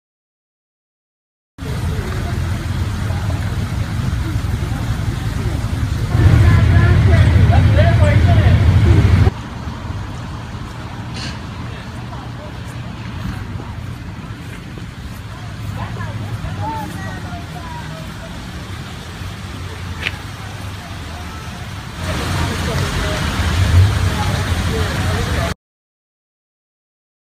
EMS: 7-year-old shot in Cleveland on Martin Luther King Jr. Drive near East 100th Street
A 7-year-old boy was shot in the shoulder on Martin Luther King Drive and East 100th Street Saturday afternoon, EMS confirmed.